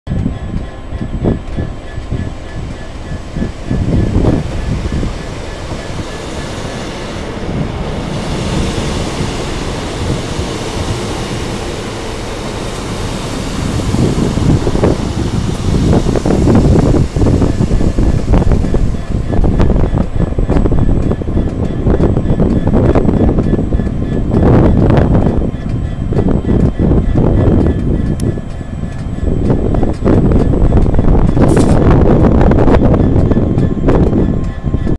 150244-150131 2V83 16/10/16 Westbury + 66067
150244 and 150131 is seen arriving at Westbury on the 16:10/16 with the 2V83 15:15 Weymouth-Bristol Temple Meads GWR service. Also in the clip is 66067 waiting its next turn of duty.